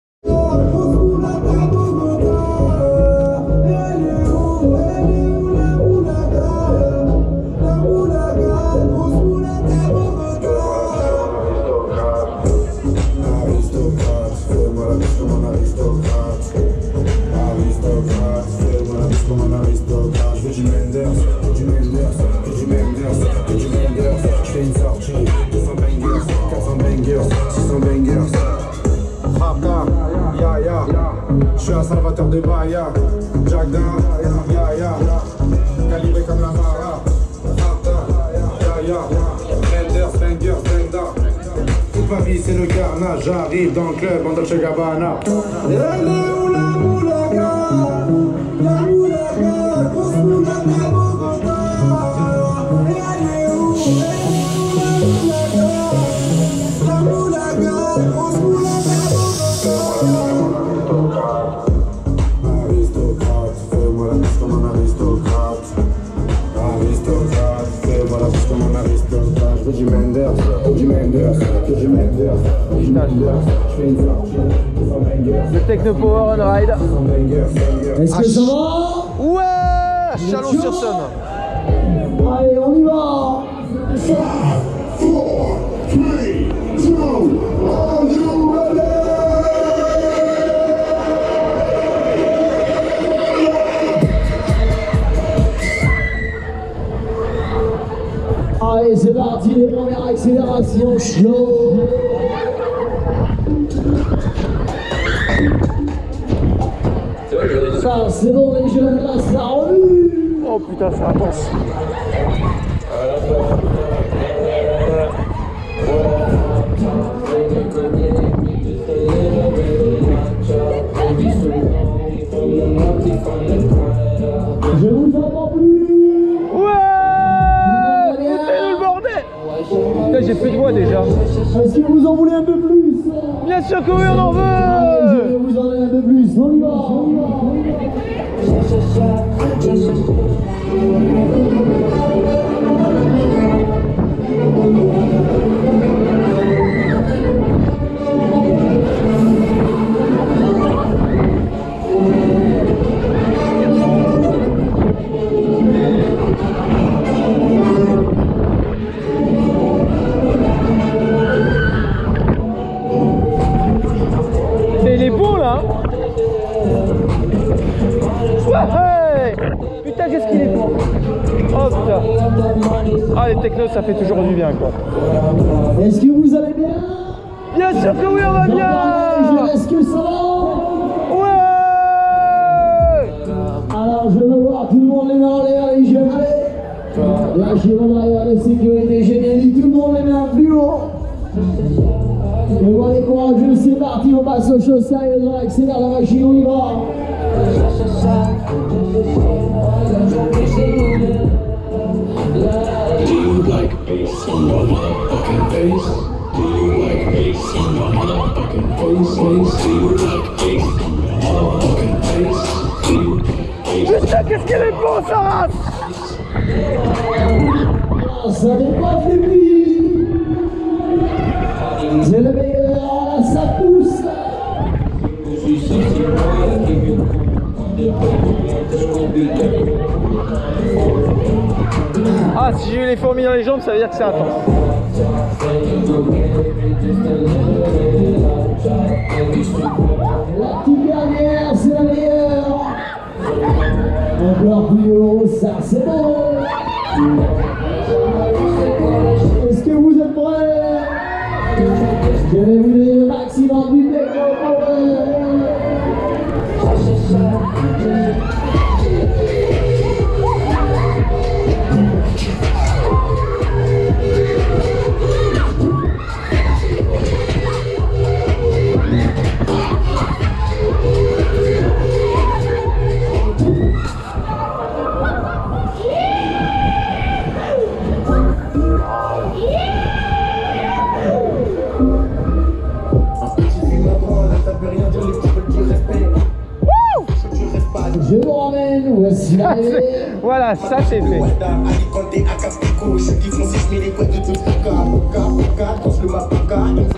Techno Power On ride (Remix, tivoli) fête foraine de Chalon-sur-Saône 2020
un super flat un techno power en plus pour moi j'en ai donc fait 5 en tout ^^